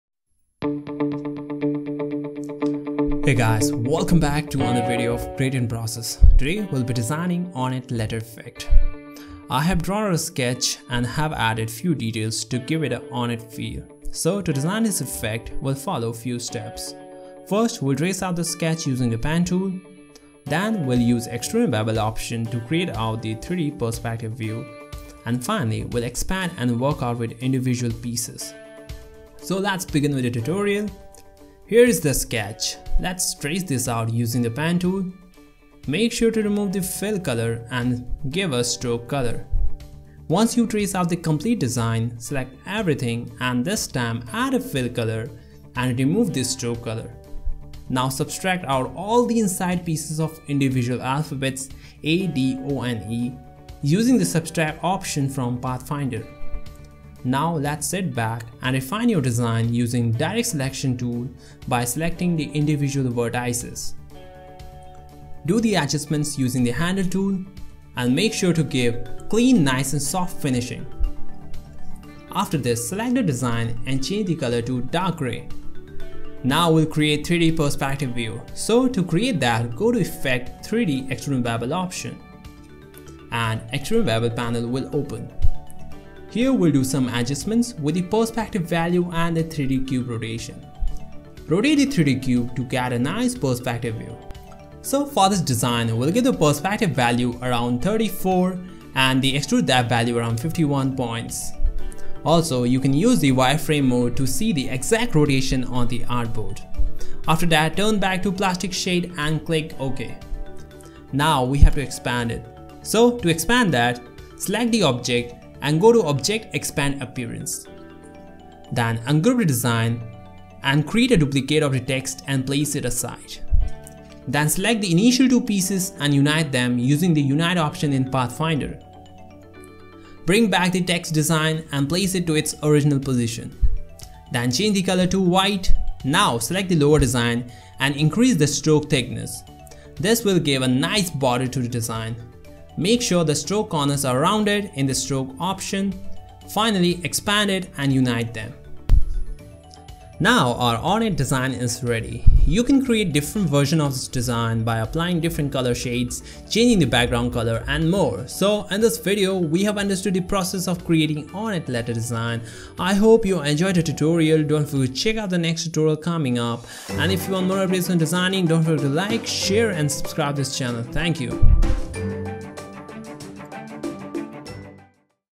Ornate Letter Design - Hard Core - Adobe Illustrator
In this Video we will be creating Ornate Letter Design.So create the effect, First trace out the sketch using the pen tool. Then using the Extrude and Bevel option, create the 3d perspective view. Finally expand the design and work out with individual pieces.
Check out the video to see the step by step process.

▲Gumroad Store!